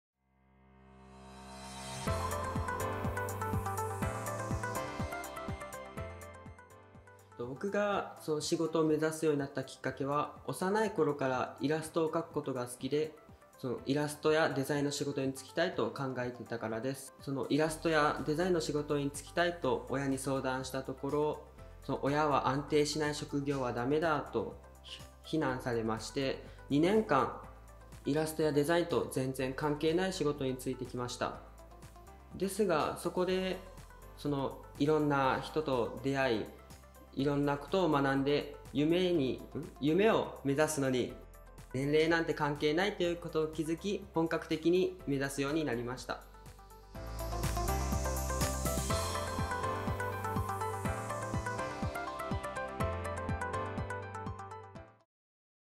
マンガ科の在校生にインタビュー！【ダイジェスト版】
学校のこと、将来のこと・・・色々と話をしてくれました。
ダイジェストでお届けします！
デザイン系のお仕事を目指している方は動画をチェック！